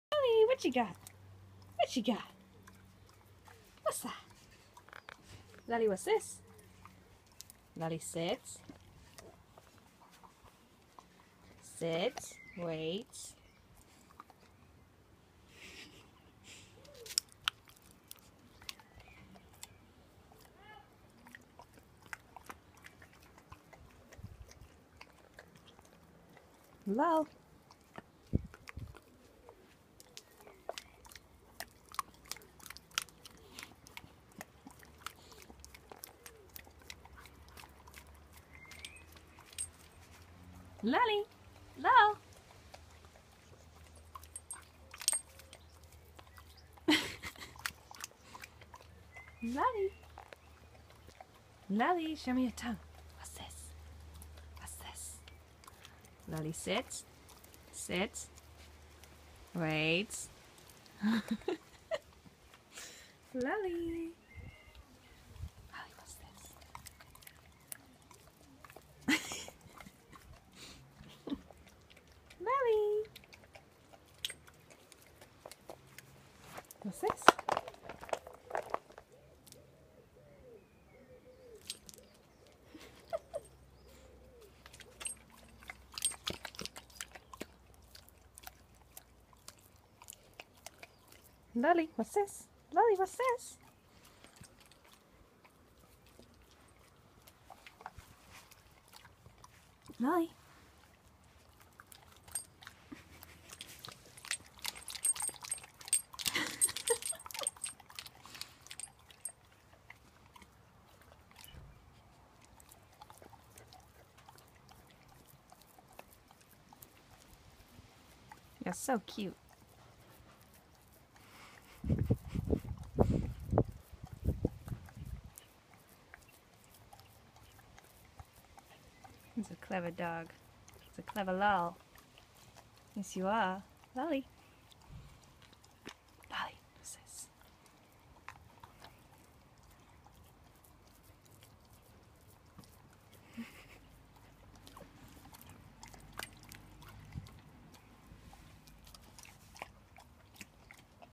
Lol the long tongued lab.
This is Lol, she just can't keep her tongue in her mouth.